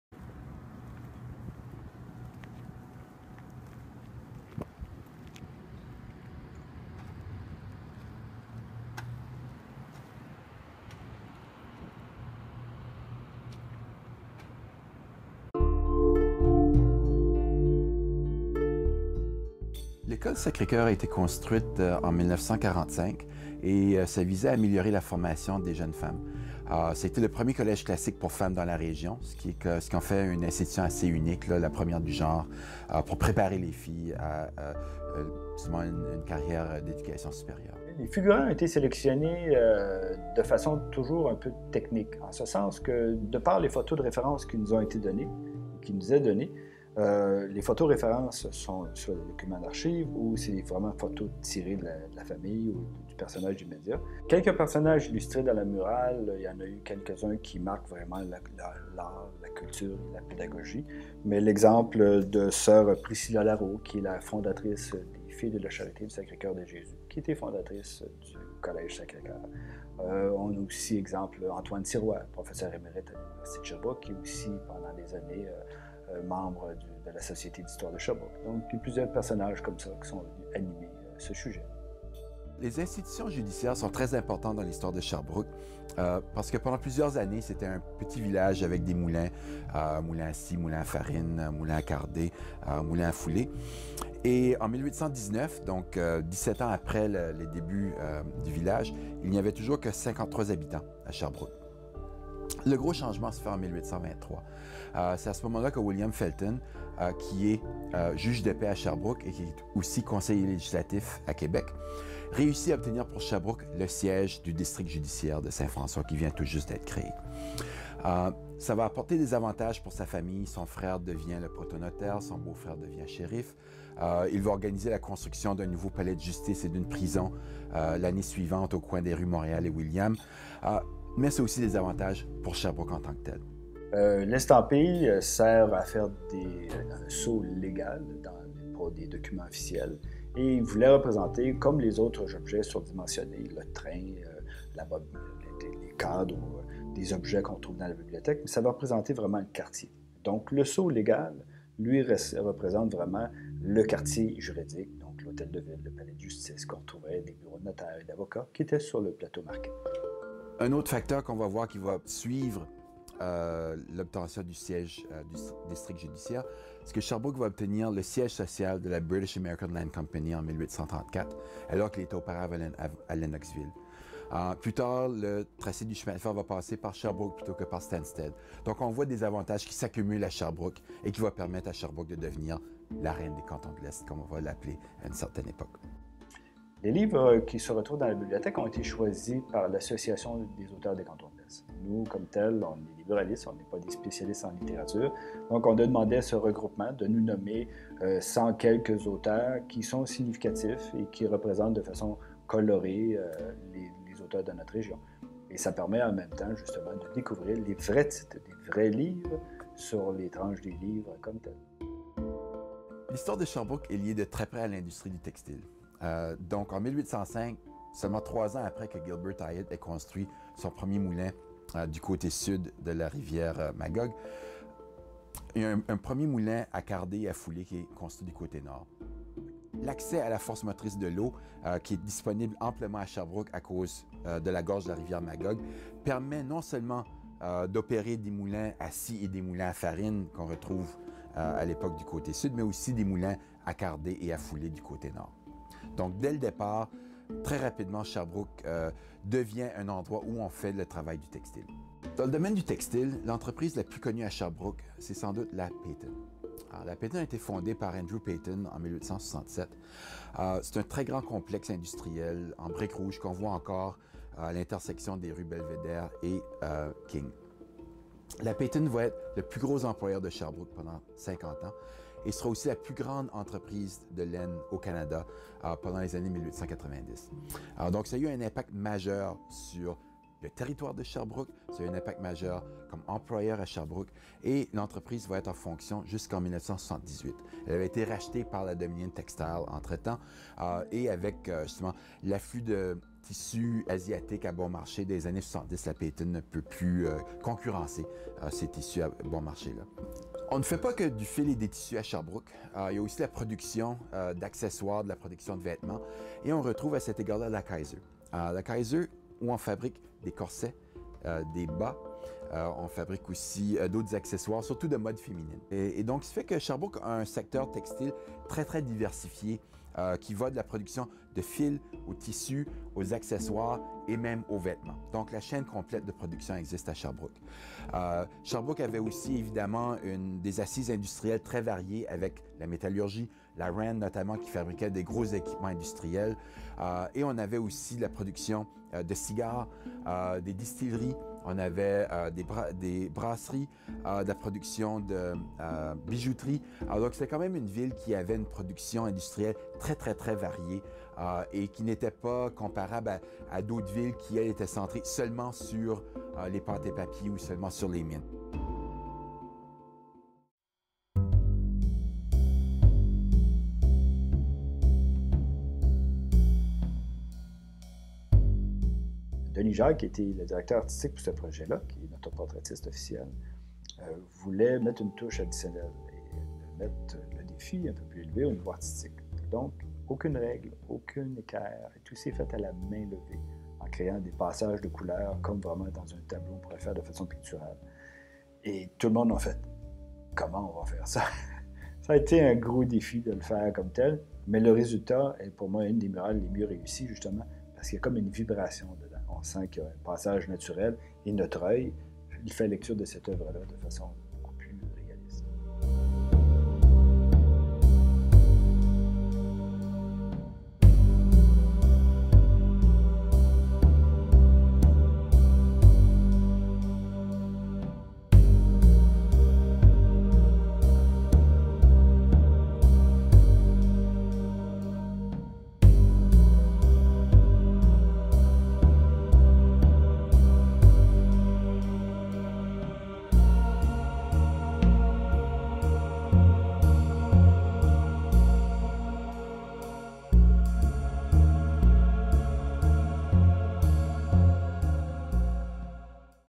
Coeur, culture et pédagogie
Capsule vidéo, réalisée par Gabrielle Thériault et Ann-Émilie Lacerte de l'Université de Sherbrooke, racontant l'histoire de la murale Coeur, culture et pédagogie.

Emplacement:
Collège du Sacré-Cœur de Sherbrooke
155, rue Belvédère nord, Sherbrooke, Québec

Cette murale influencée par les maîtres de la peinture trompe-l’œil tels que Cornelis Norbertus Gijsbrechts (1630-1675) et Giuseppe Maria Crespi (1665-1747) évoque l’histoire de ce quartier, lieu riche de culture, de littérature et de savoir.